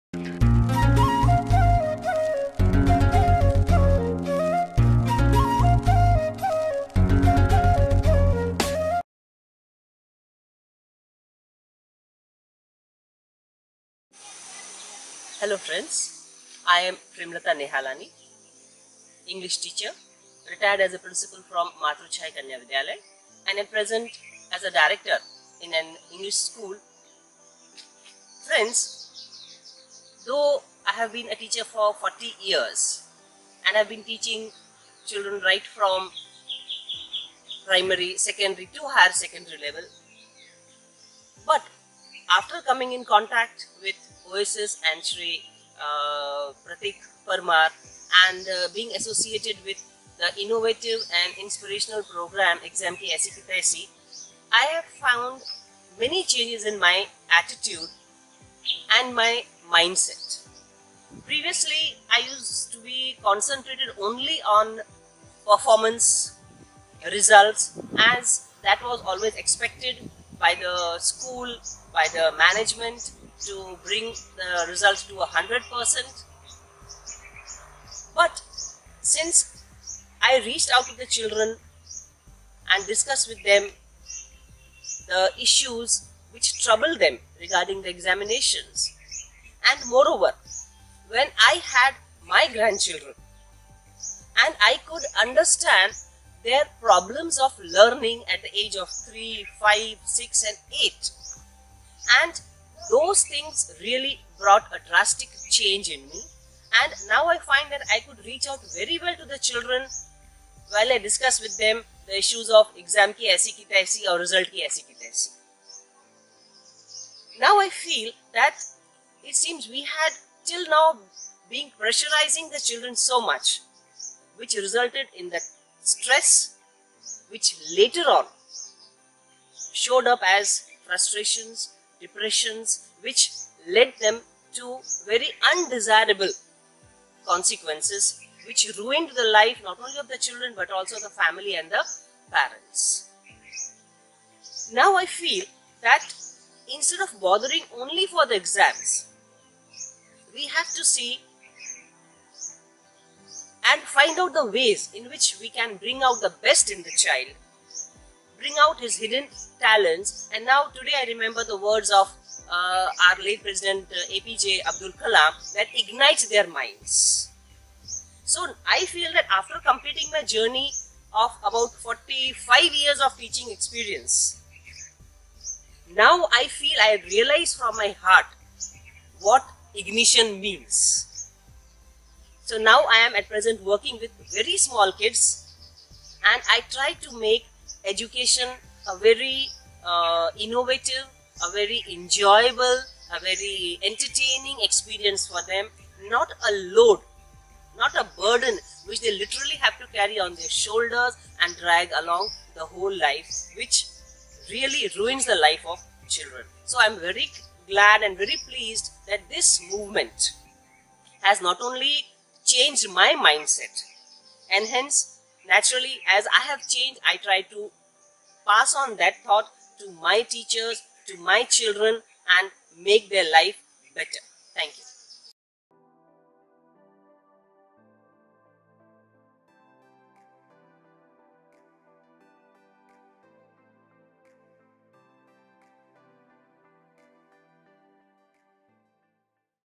Oasis: True Meaning Of Education
Premlata Nehalani from Kutch (Gujarat) shares her experience as a facilitator of Exam Ki Taisi Movement. She herself being a teacher shares the problems of children during exams.